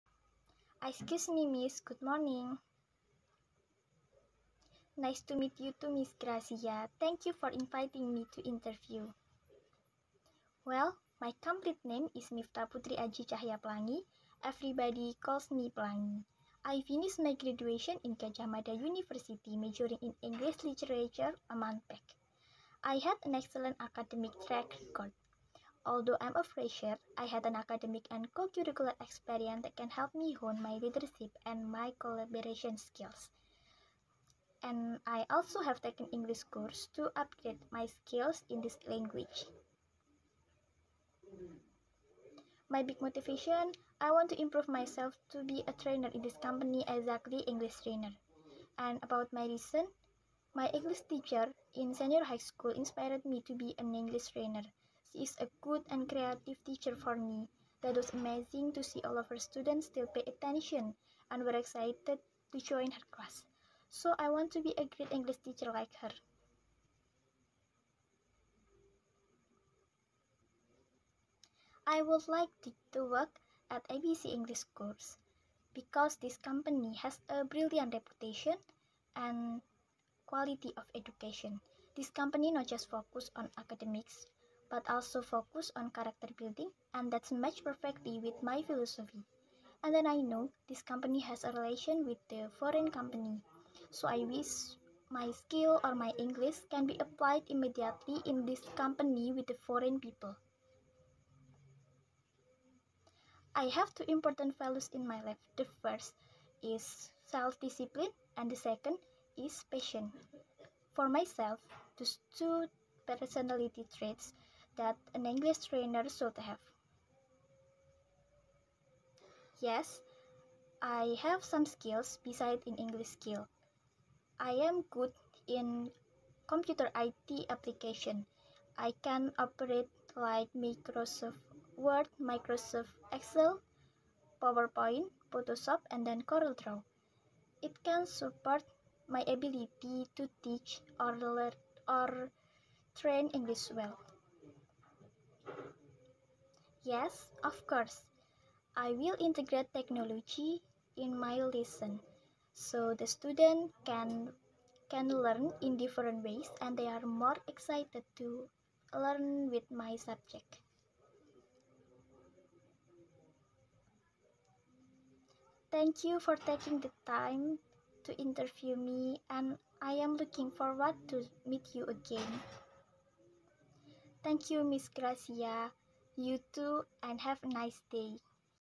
Tugas Bahasa Inggris - JOB INTERVIEW
Name      : Mifta Puteriaji Cahya Pelangi
Class       : XII MIPA 1
Number   : 14
Subject    : Bahasa Inggris